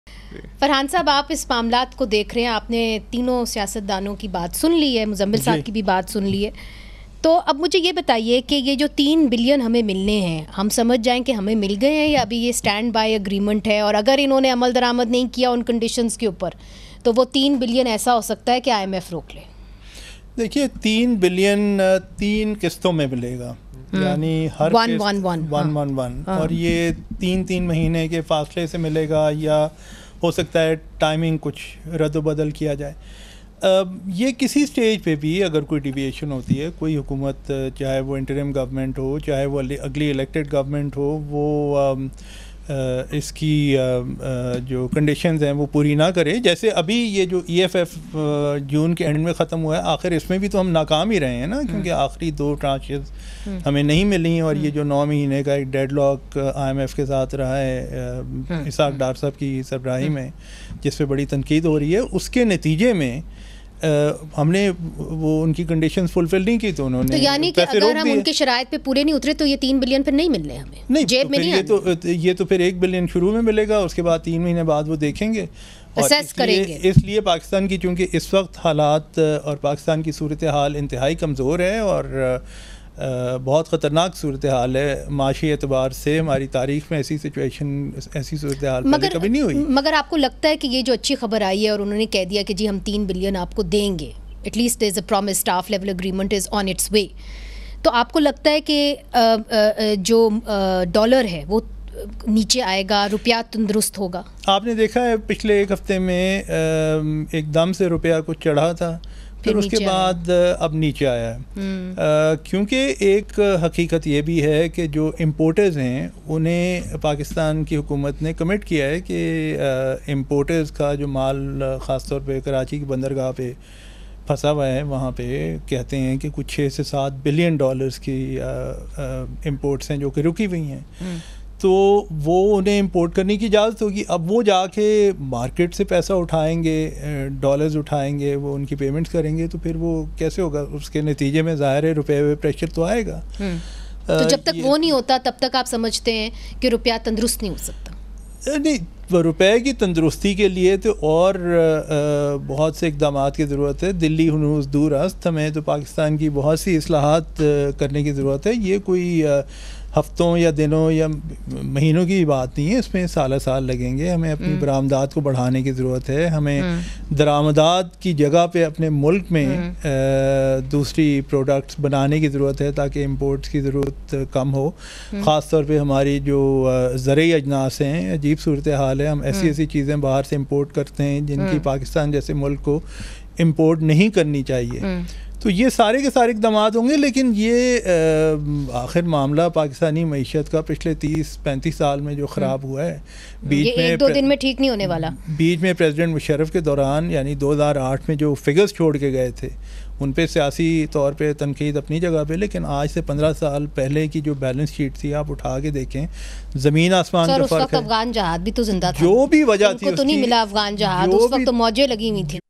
If govt does not implement IMF agreement, how much will country's economy suffer? - Aaj News
Host: Tariq Chaudhry

Guests:
- Muzzammil Aslam
- Farhan Bokhari
- Rana Afzal Khan
- Saleem Mandviwalla

Aaj TV Official:
shorturl.at/iBDNX

Aaj TV Official:
shorturl.at/iBDNX